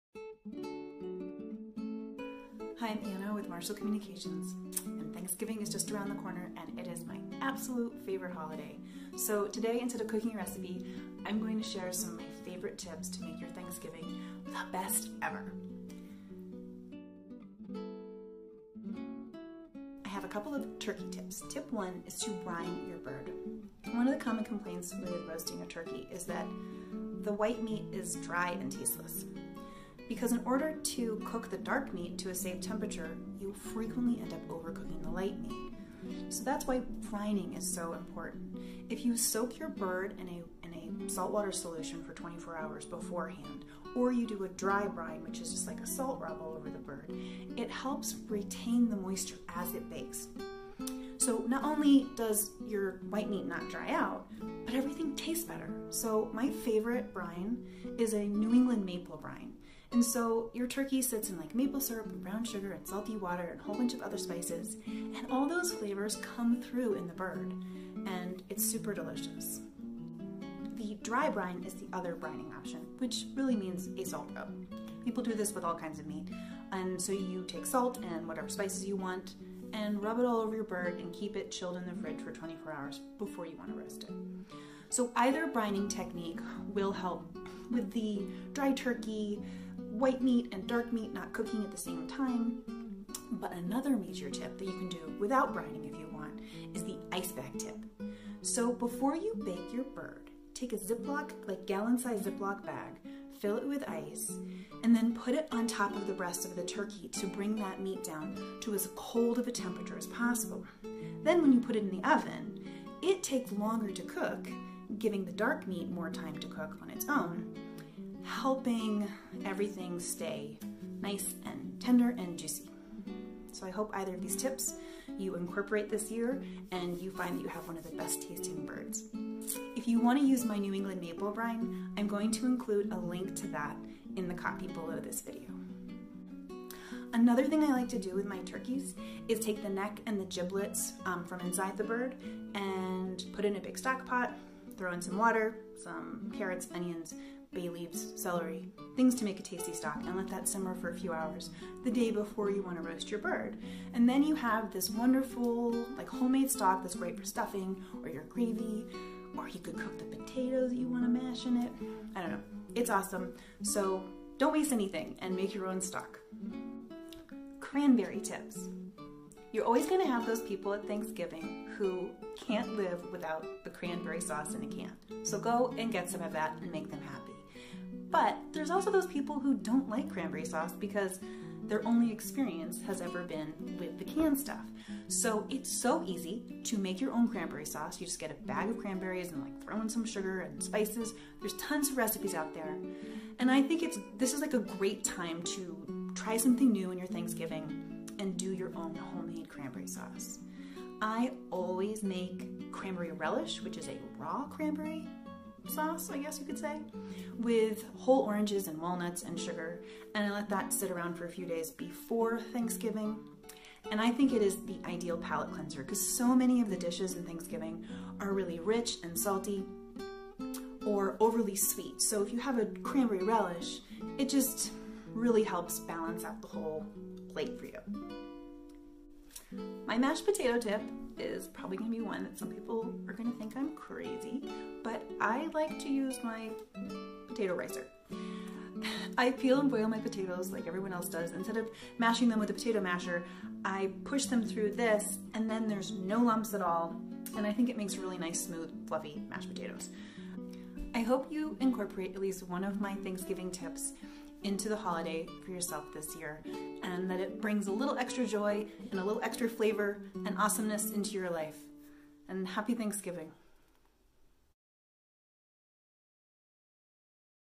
Thanksgiving Tips
New England Maple Brine:
Ingredients:
¾ pound Kosher Salt
2 cups Maple Syrup
2 Cups Brown Sugar
6 Bay Leaves
2 TBS Whole Black Peppercorns
1 TBS Crushed Red Pepper Flakes
2TBS Dry Thyme or 1 Bunch Fresh
1 TBS Dry Rosemary

Instructions:
Boil all ingredients with 1 quart of water until salt and sugar have dissolved. Remove from heat and add 3 quarts of cold water. In a brine bag, put the turkey in (giblets removed) and add brine. Let soak in cooler with ice for 24 hours. Air chill turkey for 12 hours in fridge, uncovered, before roasting.